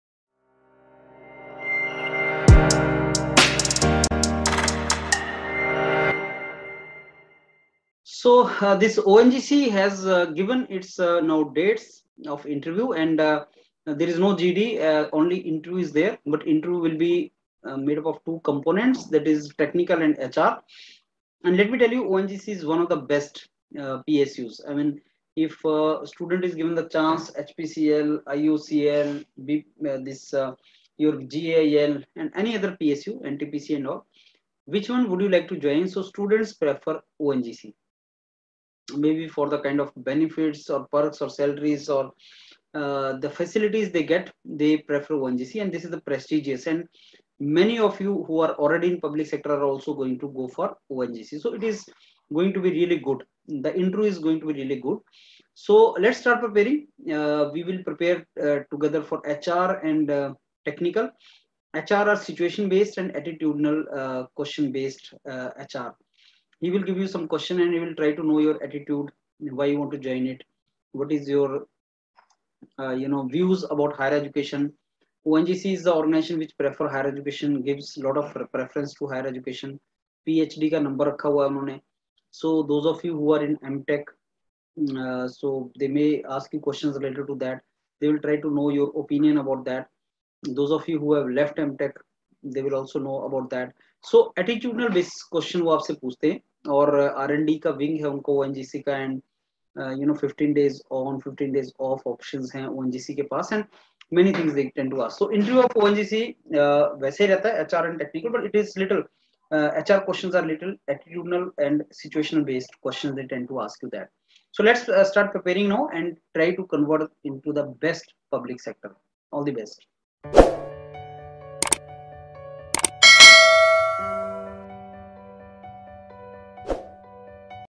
ONGC Final Interview dates announced | Do Interview preparation of ONGC with YourPedia
In this video, Dr. Vijayender has told that ONGC interview dates for various posts of AEE  Environment, Finance and Accounts Officer and Security Officer are out now against Advt No.1/2019. The shortlisted candidates will be informed separately about interview process which will be held when the situation becomes conducive with regard to prevailing pandemic.

Link for Cut off for AEE  Environment | Finance and Accounts Officer | Security Officer against Advt. No. 1/2019 (R&P) (PDF):